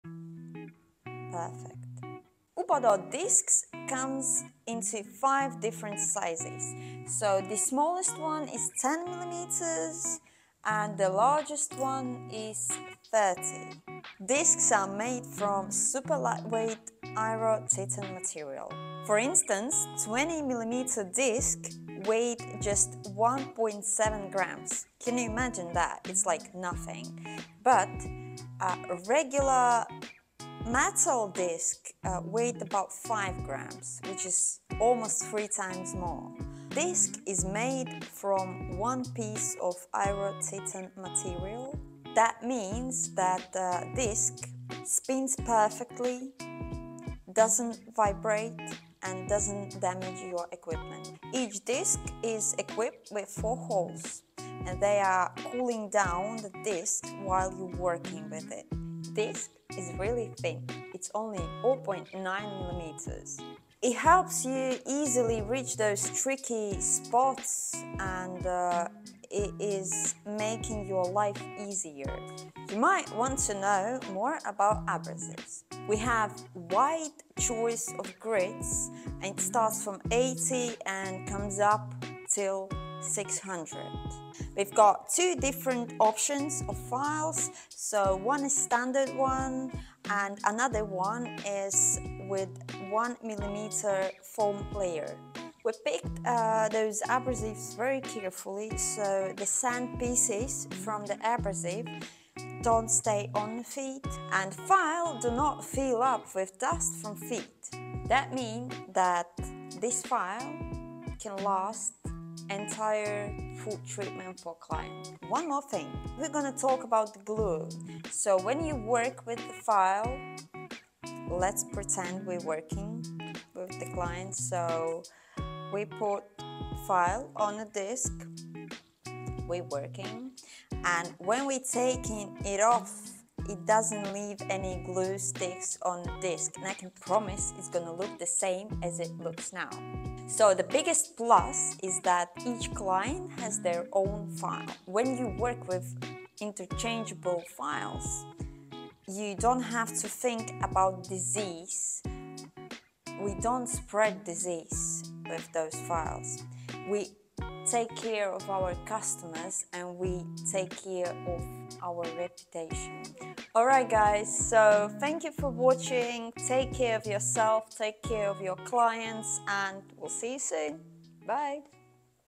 How to choose a pedicure disk?
Why settle for boring pedicures when you can have a foot spa experience? Our U-Premium Podo Discs are the perfect way to pamper your clients’ feet. With five different diameters to choose from, you can customize each pedicure to perfection. Trust us, your clients will be walking on cloud nine!